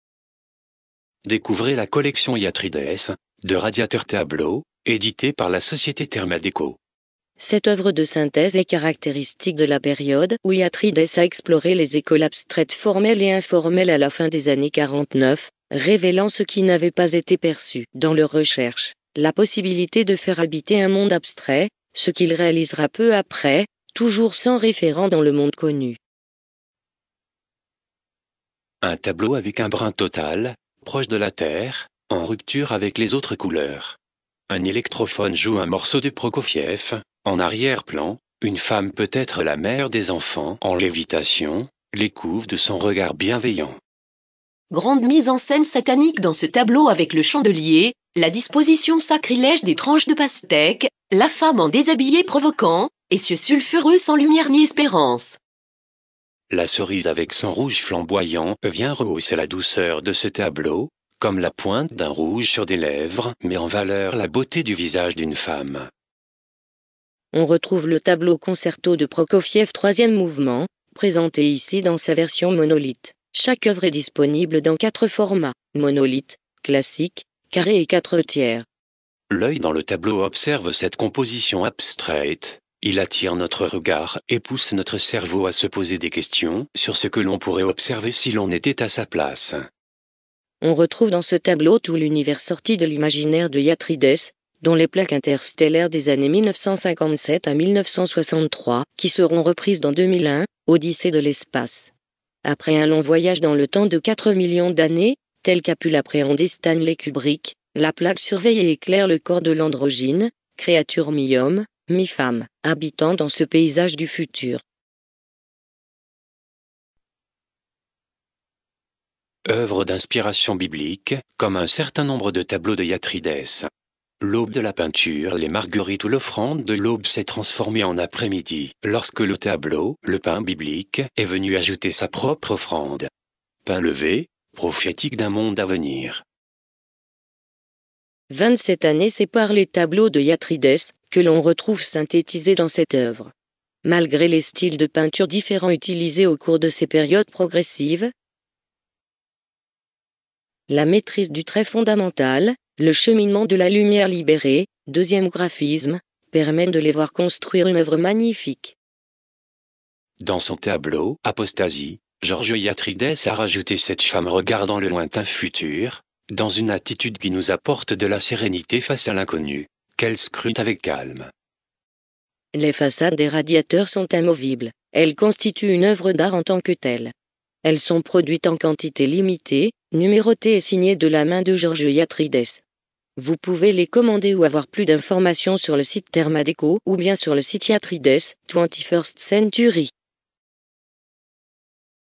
Radiateurs tableaux collection Yatridès
Présentation de la collection de radiateurs tableaux Thermadéco by Yatridès. Ces façades de radiateurs amovibles sont fabriquées en séries limitées, numérotées et chaque pièce est signée de la main de l'artiste, elles sont issues des oeuvres hyalotroniques originales de Georges Yatridès et n'ont encore jamais été diffusées auparavant auprès du public.